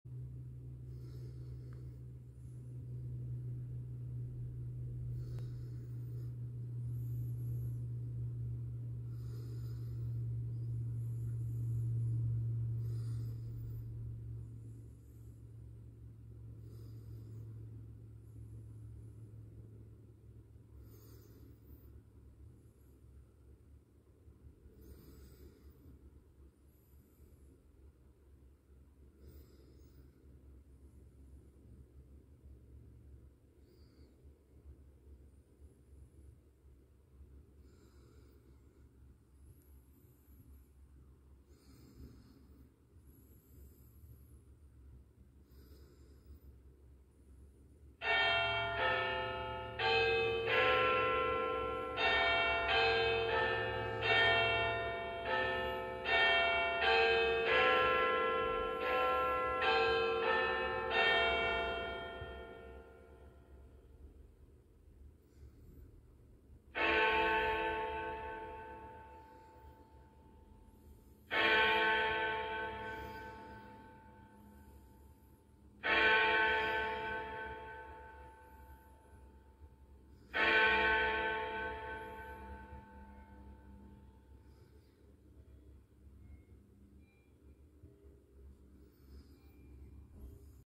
Arduino project Big Ben Chiming Clock
I recently learned about Arduino and decided to make a big ben hourly chiming clock on a Nokia 5110 LCD using an Arduino Nano.